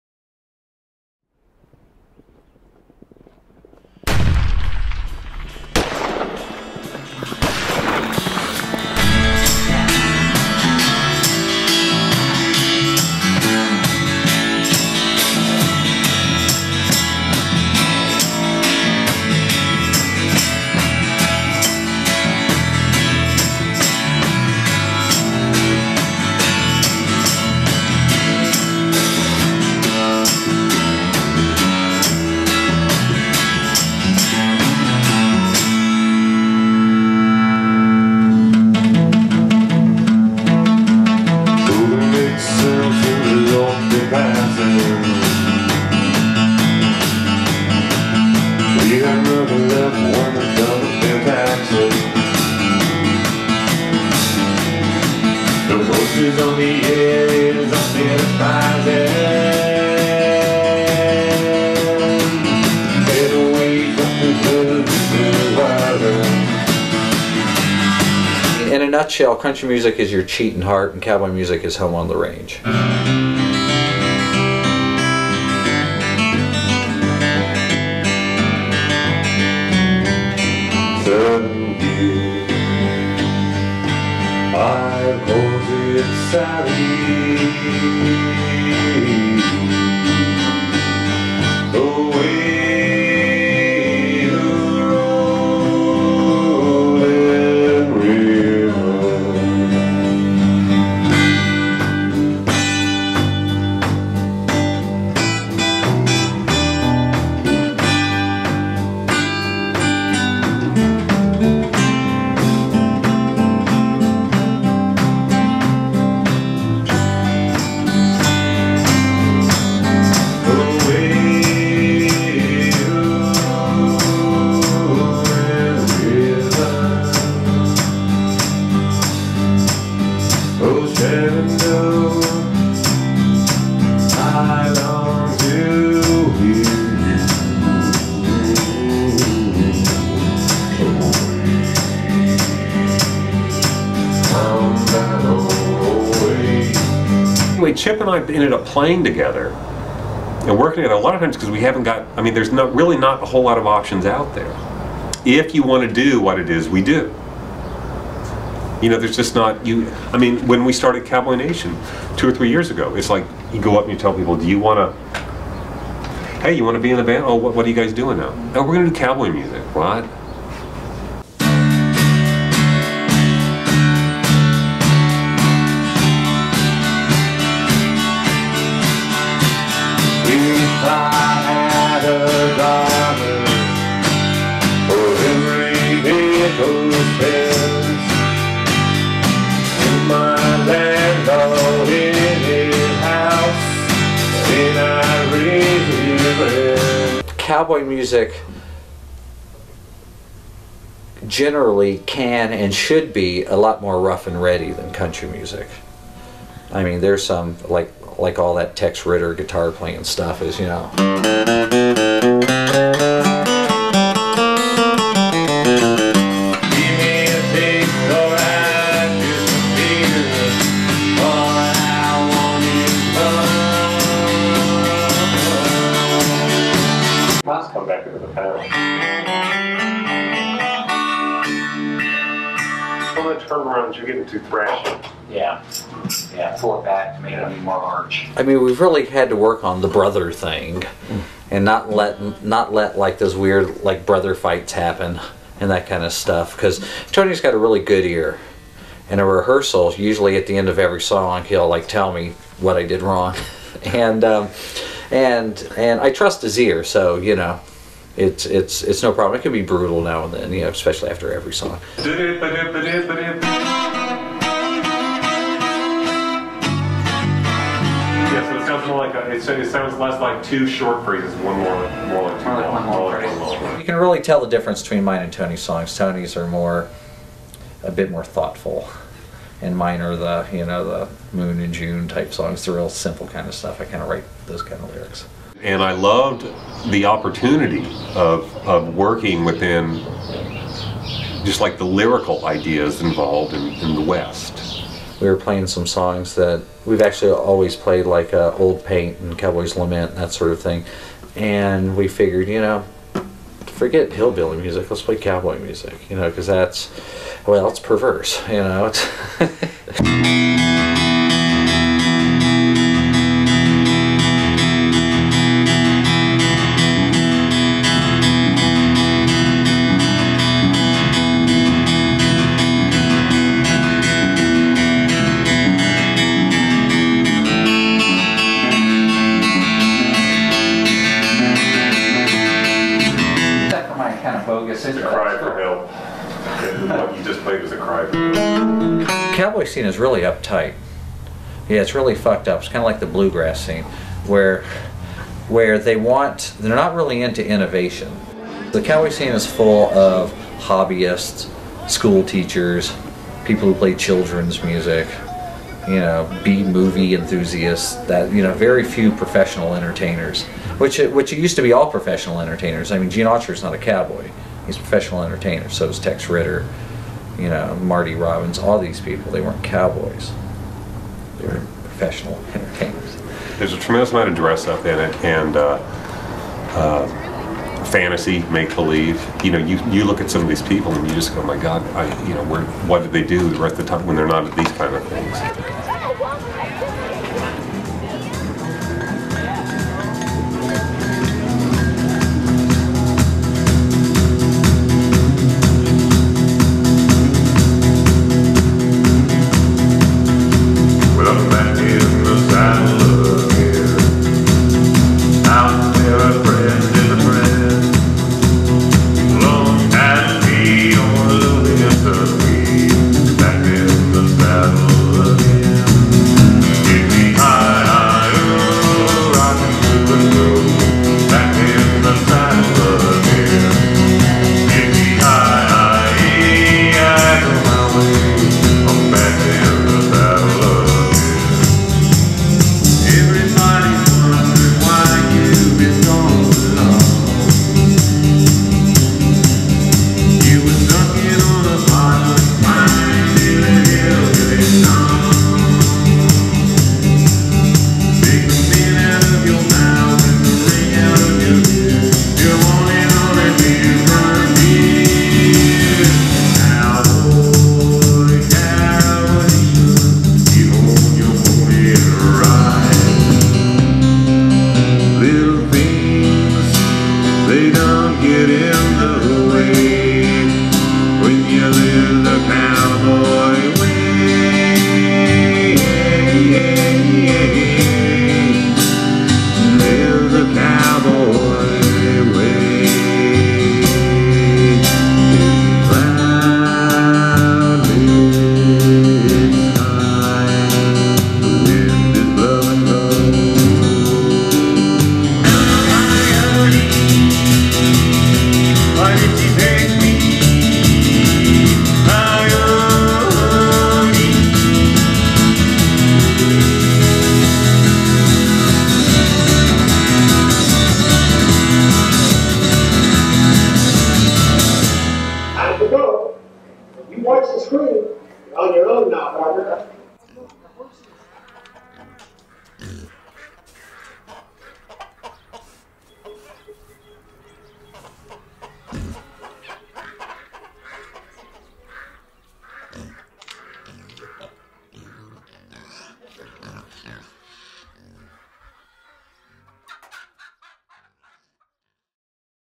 Cowboy Nation
A short documentary about the great cowboy band Cowboy Nation, featuring Tony & Chip Kinman.  Directed by Jordan Ellis